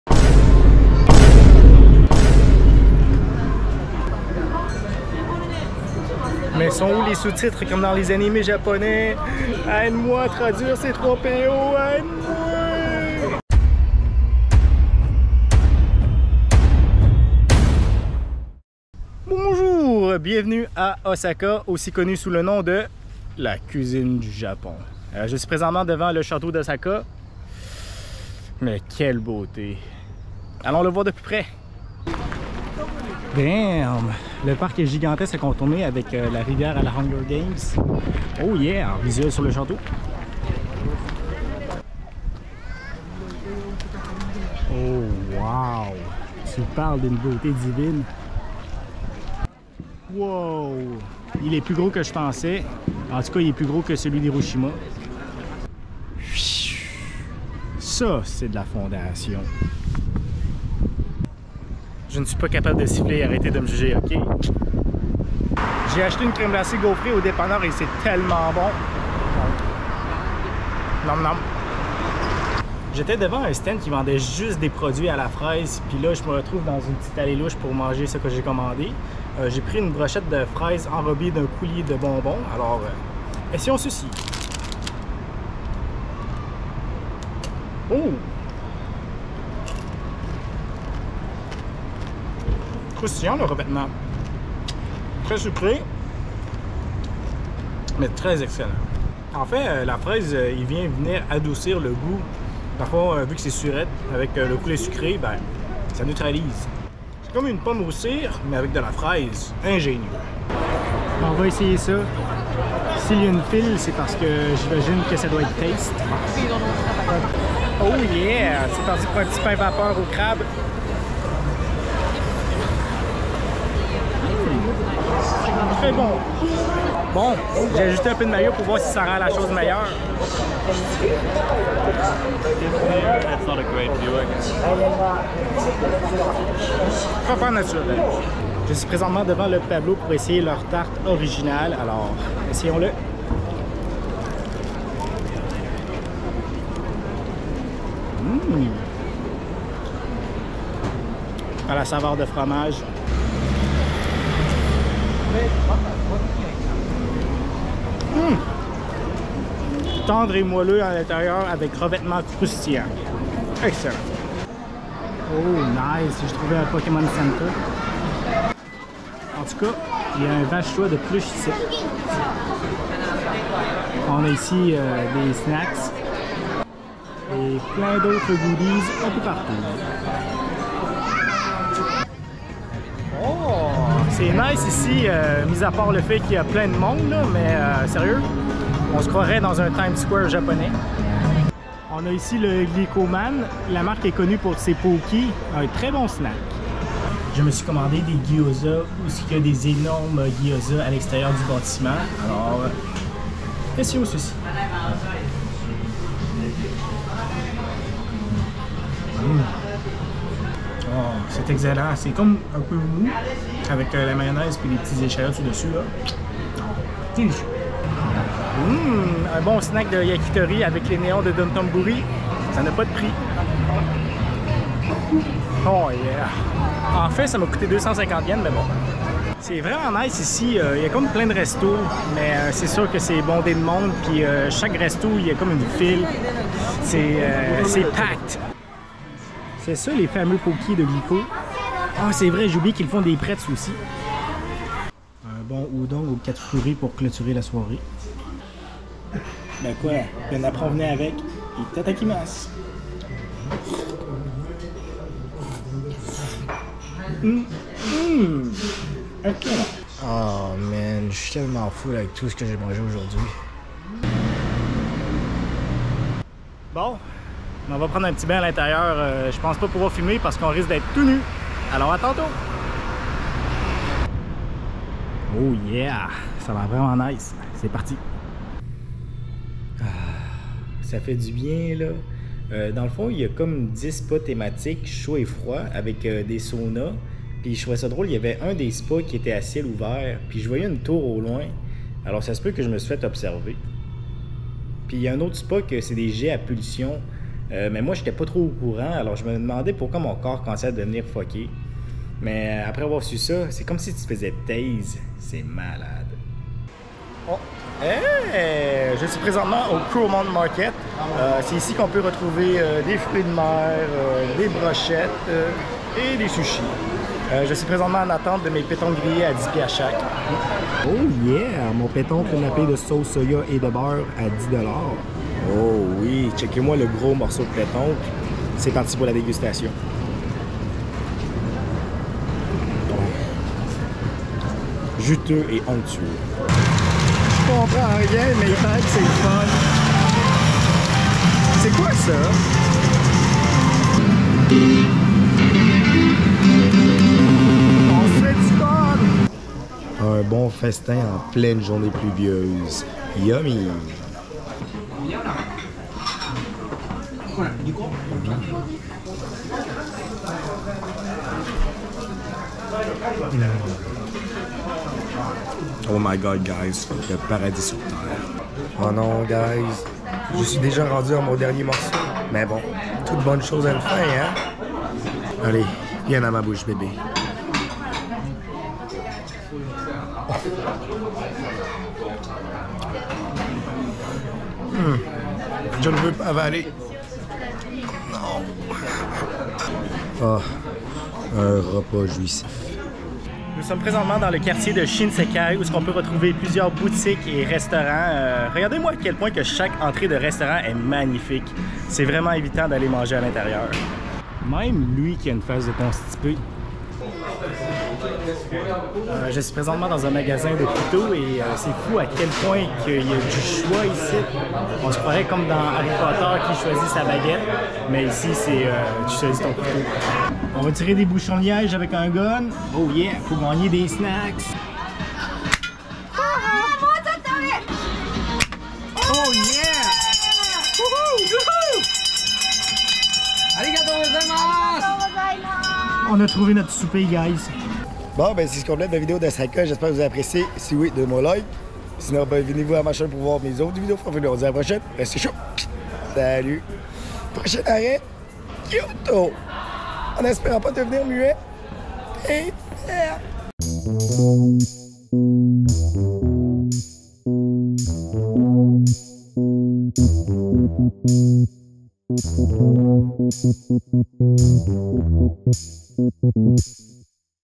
LA BONNE BOUFFE D'OSAKA!
Nous voilà à Osaka afin de découvrir le château d'Osaka, Dotonbori et Shinsekai tout en mangeant la bonne bouffe sur place!